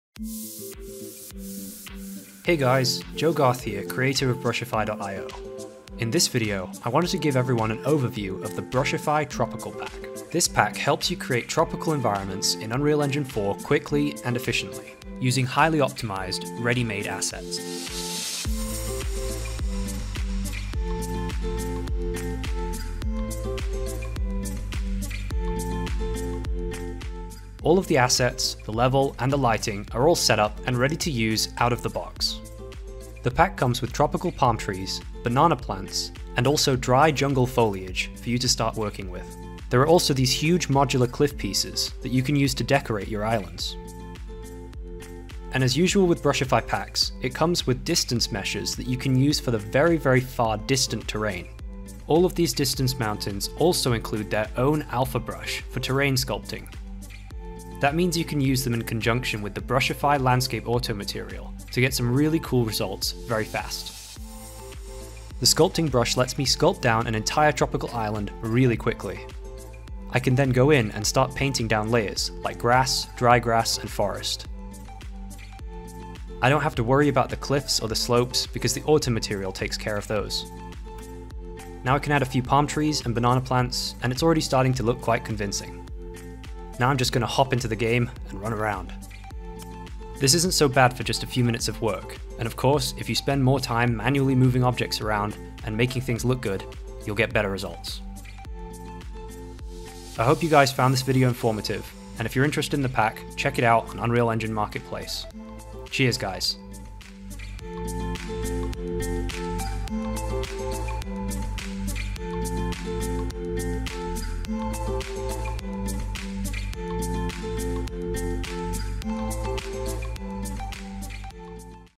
Brushify - Build a Tropical Island scene in UE4 (Available on Unreal Marketplace)
PC specifications:
CPU - AMD Threadripper 1950x - US UK
64GB Ram - G.Skill Ripjaws V - US UK
1080TI - US UK

Create massive open worlds in Unreal Engine 4 using Brushify!

All content in this video is included in the pack.